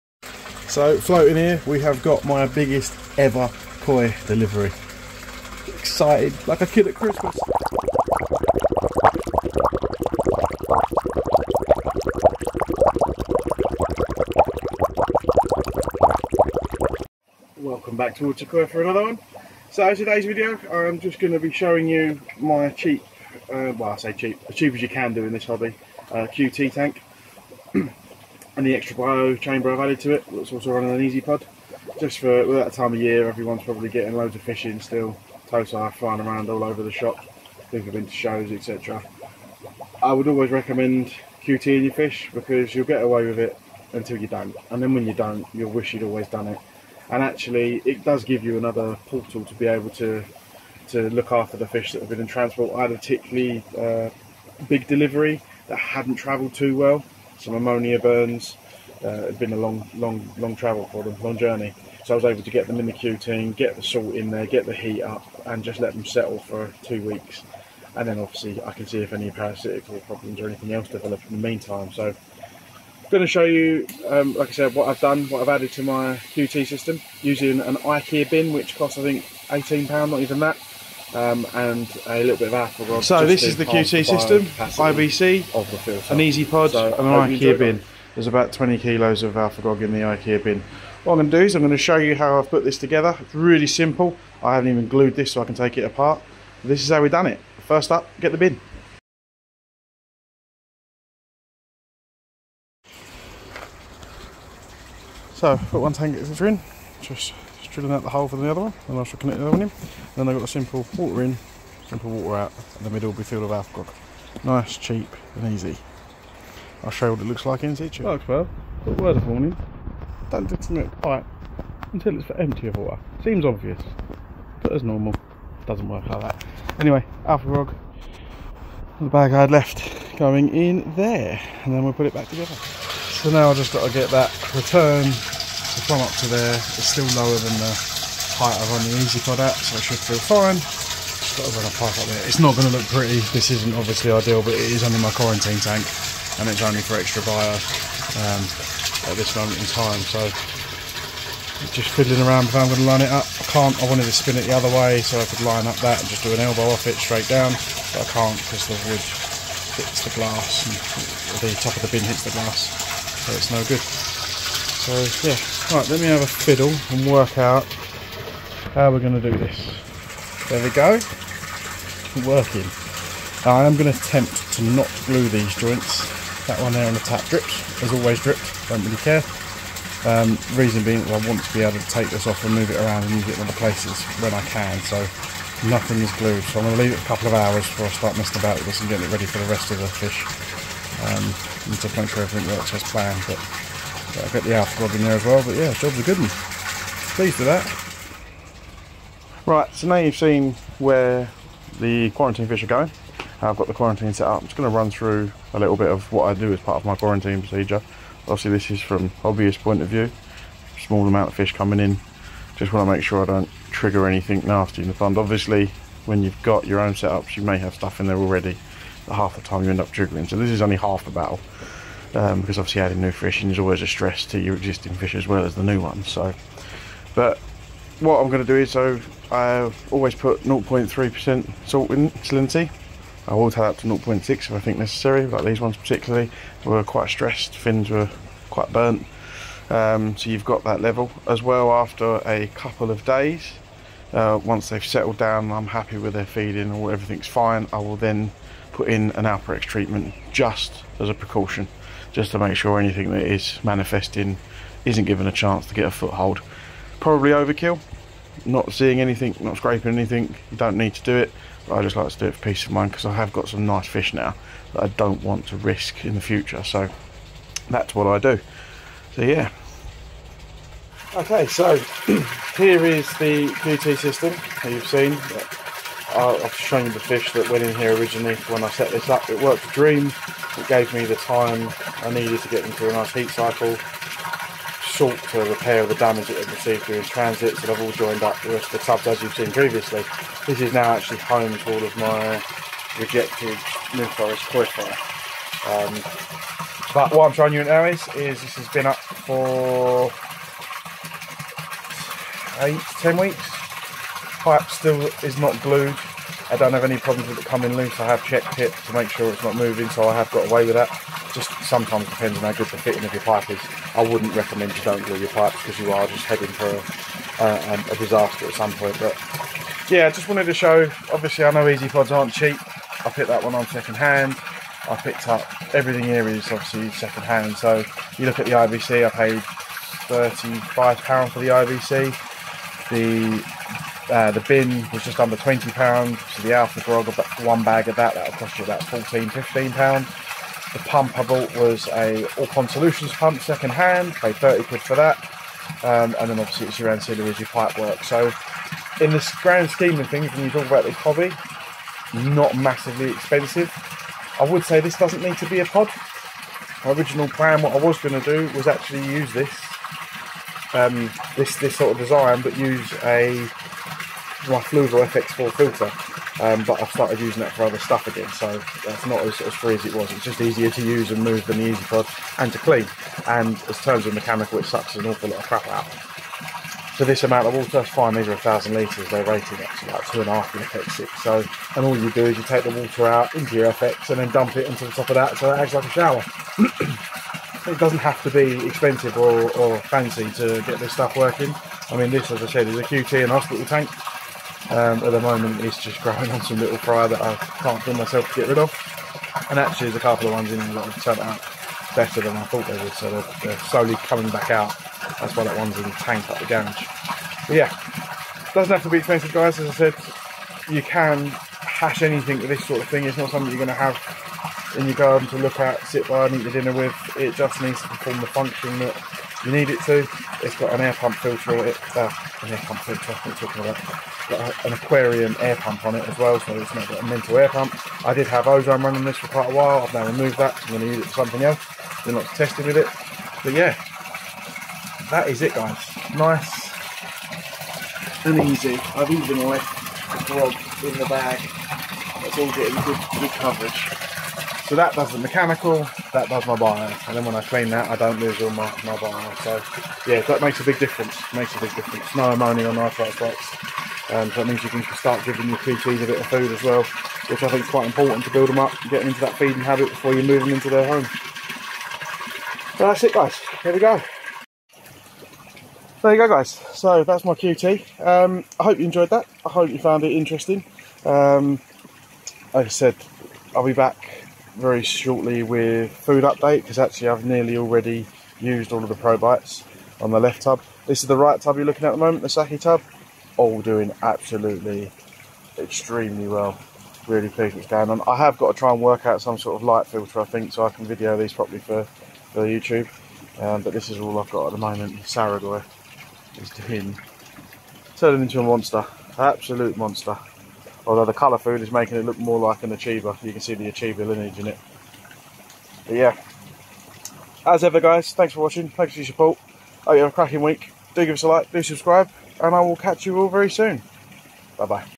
My Small Quarantine set up : DIY IKEA filter
Join us to today as we look at my Quarantine build and how I take my own fish through my own quarantine setup. I also show you how I used an IKEA bin to build a £15 Filter to help the BIO loads